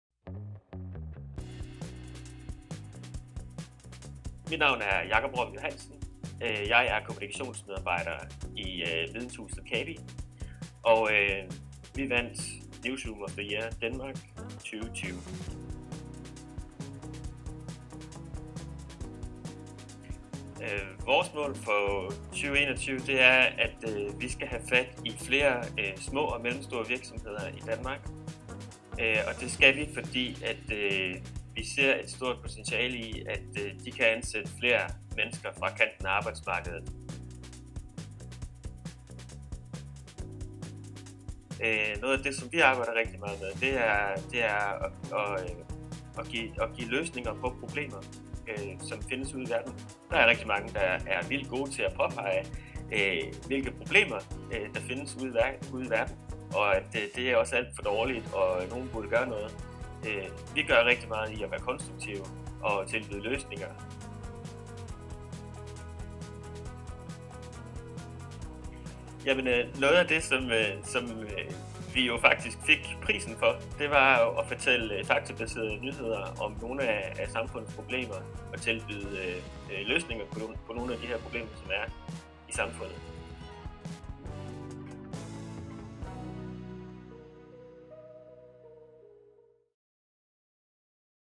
Interview with Jakob Rom Johansen, Cabi - Newsroom of the Year Denmark
Interview with Jakob Rom Johansen from Cabi, winner of Newsroom of the Year Denmark at Nordic Digital PR Awards 2020.